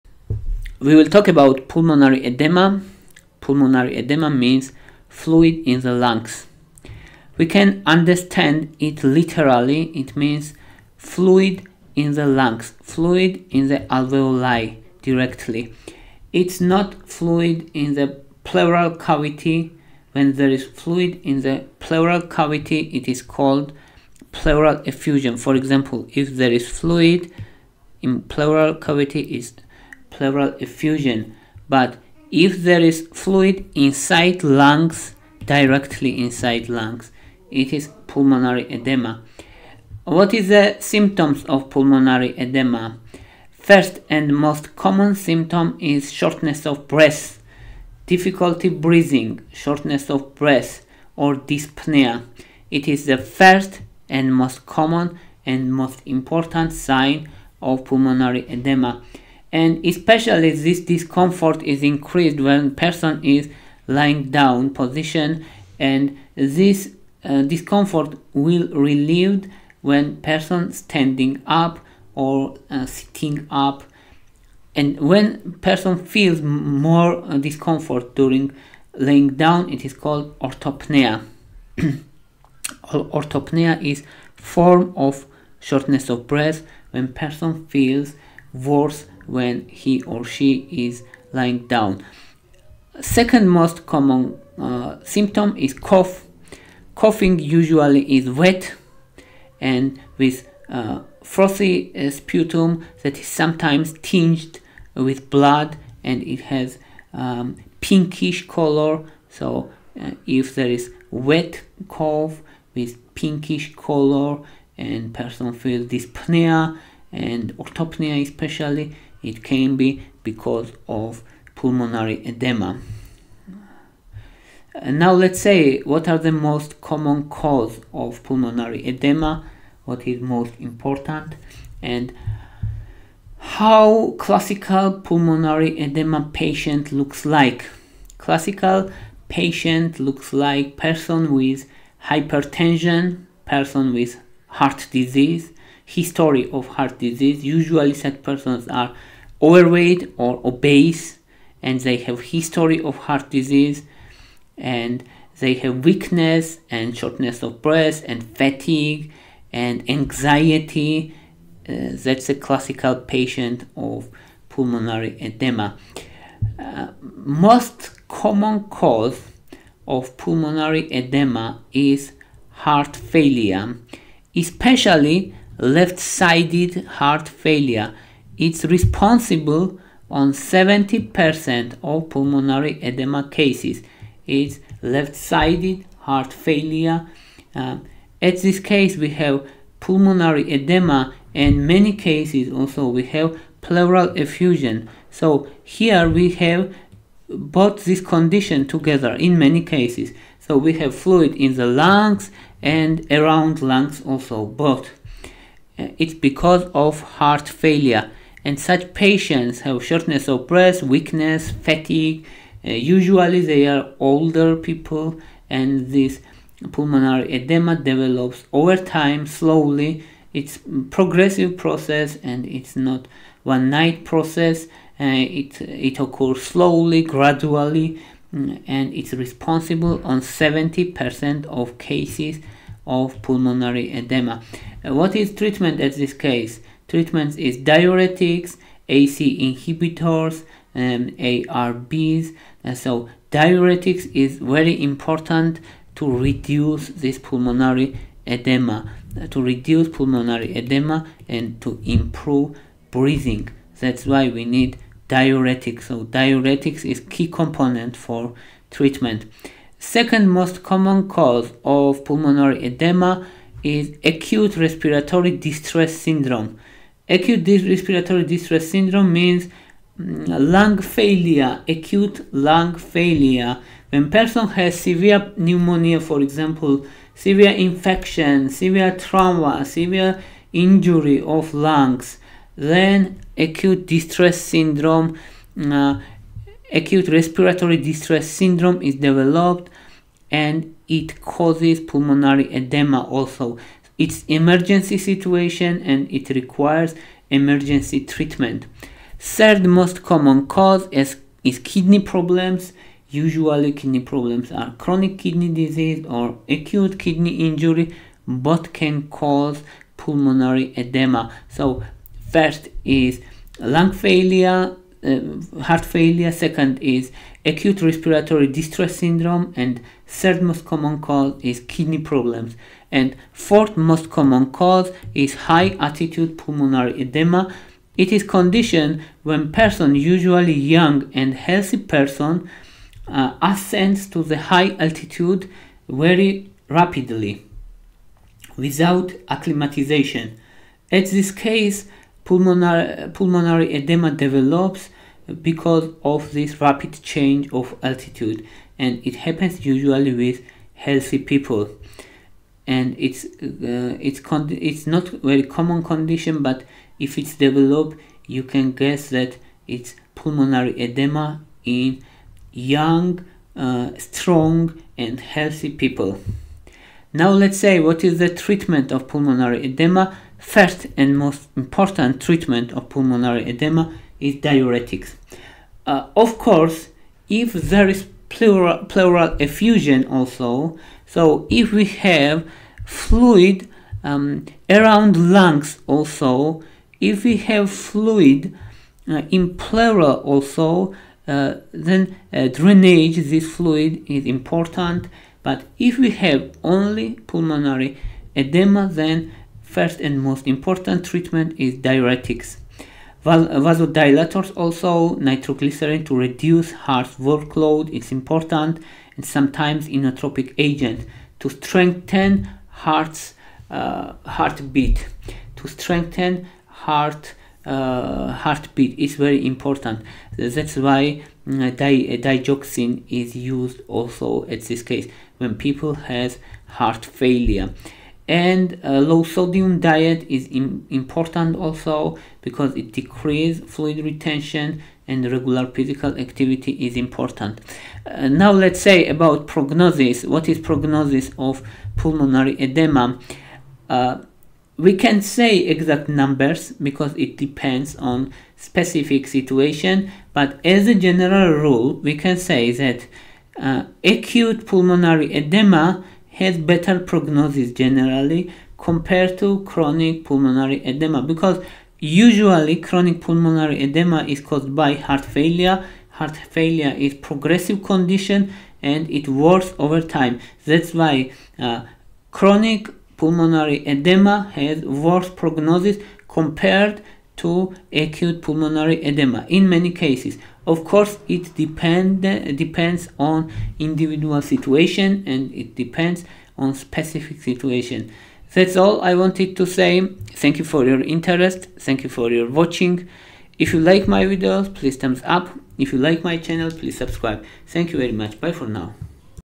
Understanding Pulmonary Edema: Symptoms and Causes
Pulmonary Edema Definition: Fluid accumulation in the lungs, leading to breathing difficulties.
Primary Symptom - Shortness of Breath: Especially noticeable when lying down (orthopnea).
Comfort in Upright Position: Symptom relief when standing or sitting due to fluid distribution.
Cough with Frothy Sputum: Often pinkish due to blood tinges.
Typical Patient Profile: Older individuals with a history of heart disease or hypertension, often overweight or obese.
Diagnostic Tools: Usage of X-rays and ultrasounds in identifying pulmonary edema.
Leading Cause - Heart Failure: Particularly left-sided heart failure, responsible for about 70% of cases.
Heart Failure Factors: Various heart conditions like vascular disease, heart valve disease, hypertension, and cardiomyopathy.
Secondary Cause - Acute Respiratory Distress Syndrome (ARDS): Responsible for 10-15% of pulmonary edema cases.
Kidney Problems as a Cause: Chronic kidney disease or acute kidney injury accounting for 5%-10% of cases.
High Altitude Pulmonary Edema (HAPE): Affects rapid ascenders to high altitudes, more common in young, healthy individuals.
Treatment Necessity: Pulmonary edema typically requires intervention, not self-resolving.
Common Treatments: Include diuretics, vasodilators, inotropic agents, and lifestyle changes like a low sodium diet and regular exercise.
Non-standard Treatment: Direct fluid suctioning from lungs is generally not feasible or recommended.
Prognosis Variability: Heavily dependent on the underlying cause; cardiogenic pulmonary edema often has a worse prognosis than noncardiogenic.